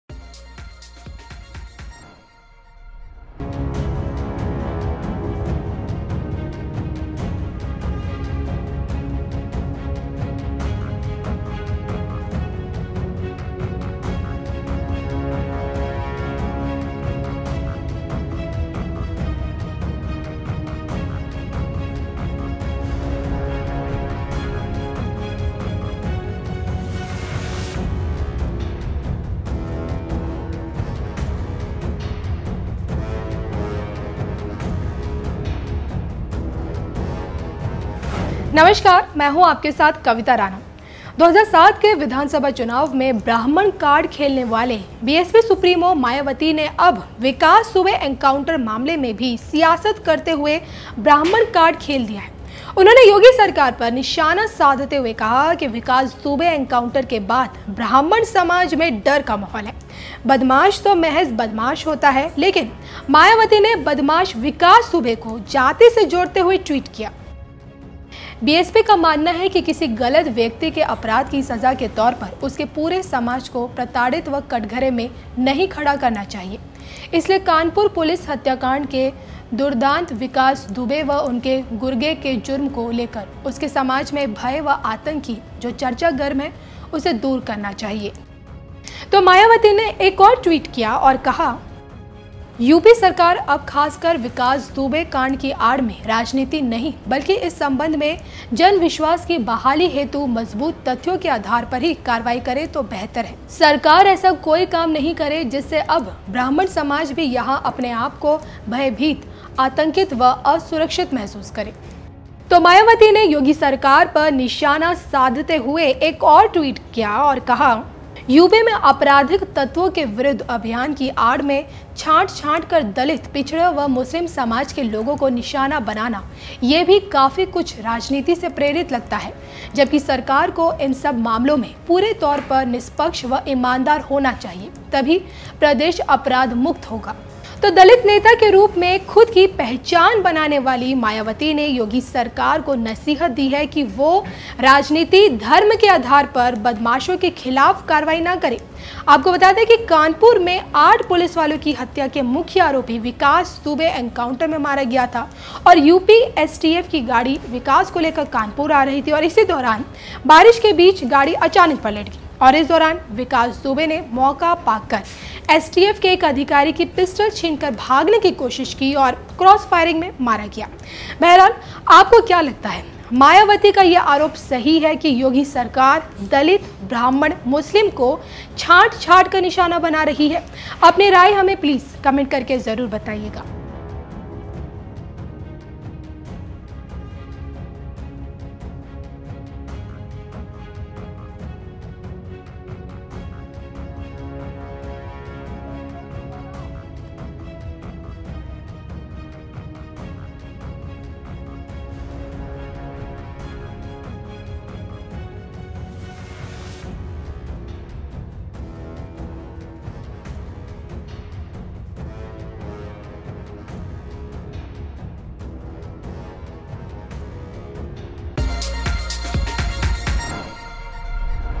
Mayawati बोलीं Vikas Dubey Encounter से ब्राह्मणों में है डर का माहौल !
2007 के विधानसभा चुनाव में ब्राह्मण कार्ड खेलने वाली बीएसपी सुप्रीमो मायावती ने अब विकास दुबे एनकाउंटर मामले में भी सियासत करते हुए ब्राह्मण कार्ड खेल दिया है... उन्होंने योगी सरकार पर निशाना साधते हुए कहा है कि विकास दुबे एनकाउंटर के बाद ब्राह्मण समाज में डर का माहौल है...